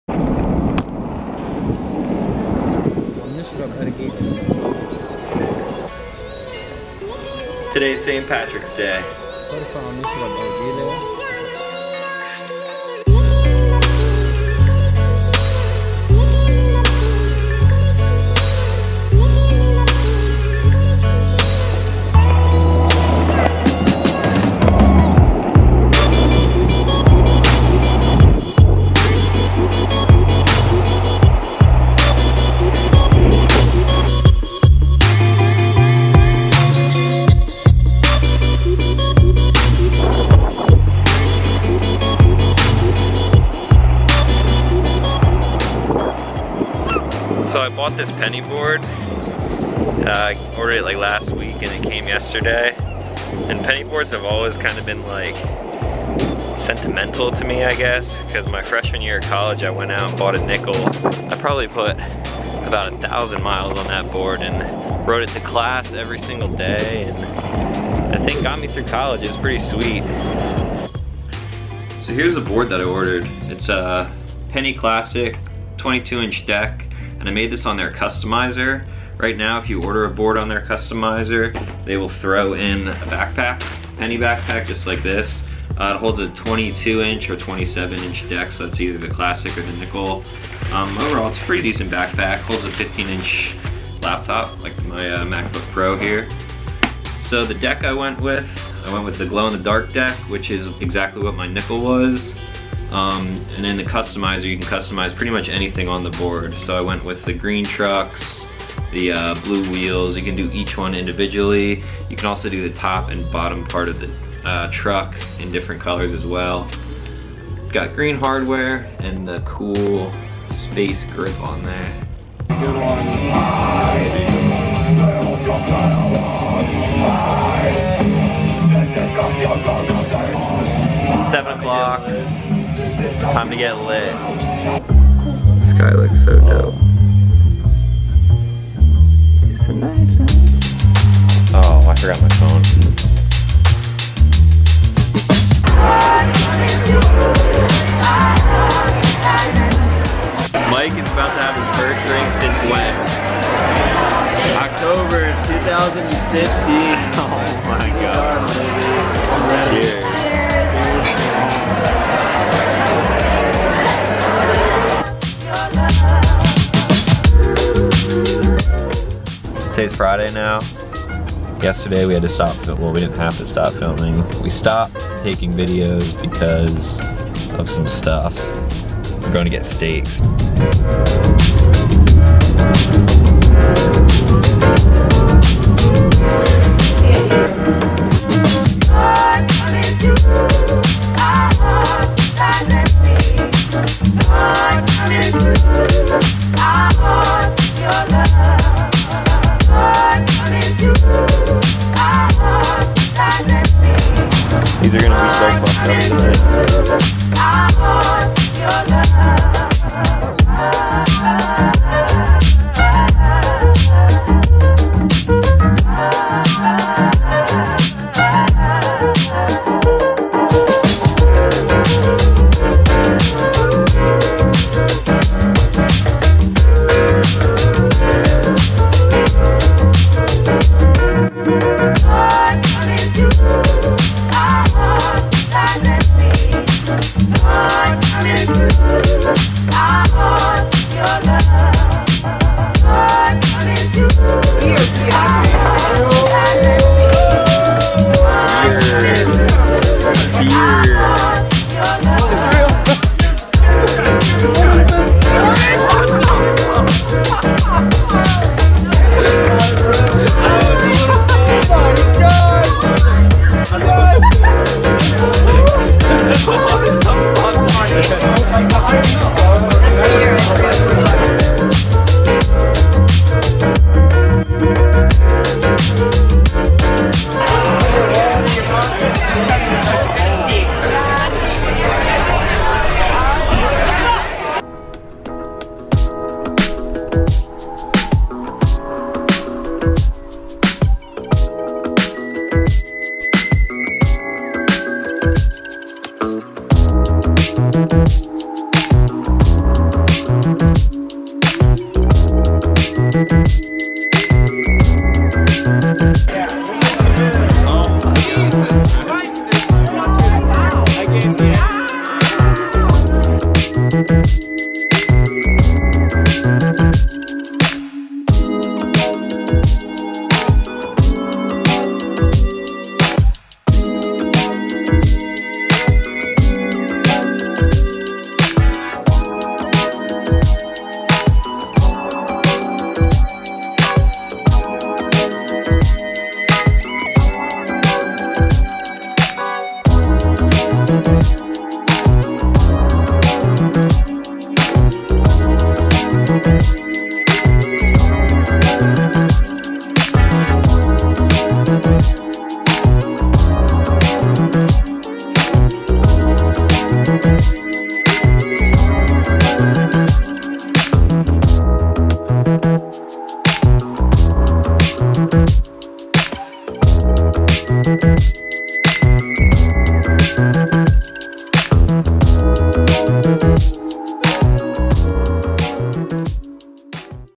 Custom Penny Board!
Vlog #008
Skating around Boston Harbor and celebrating St. Patricks day